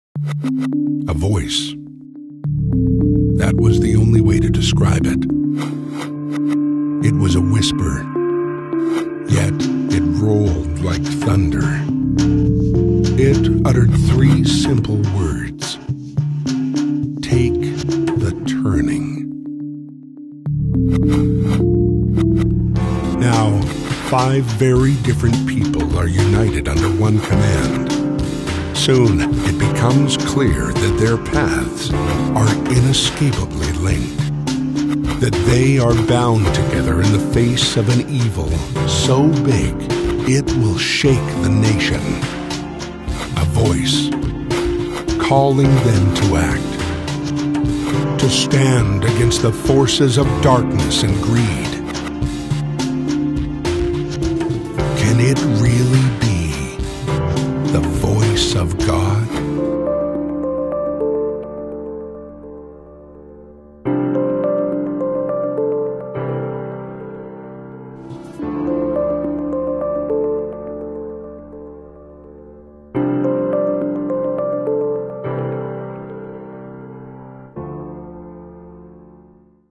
The Turning by Davis Bunn
The message was unexpected but instantly recognizable.

A voice resonated from a distance and somehow from within.  Against all earthly logic, it carried a divine command.  And five very different people knew they were summoned to obey.

Their actions were demanding, but not particularly grand.  Only later would they see a pattern emerge - one that links their tasks together and comes to challenge the cultural direction of the nation.  They realize that one small personal response unveiled a new realm of moral responsibility.  And this affirmation of everyday hope captures the attention of millions.

But power and money are at stake.  Malicious elements soon align themselves to counter the trend.  To succeed they must also undermine its source.  Can we really believe that God speaks to people today?  Surely this must be dismissed as superstition or delusion.  These well-intentioned but misguided individuals should not be allowed to cast our society back into the Dark Ages.

The public debate and media frenzy place an unprecedented spotlight on knowing and doing God's will.  The five encounter threats, but try to remain steadfast in their faith.  Had God indeed imparted wisdom on selected individuals?  Is this sweep of events part of his divine purpose?

The movement may herald a profound renewal - one that some are calling The Turning...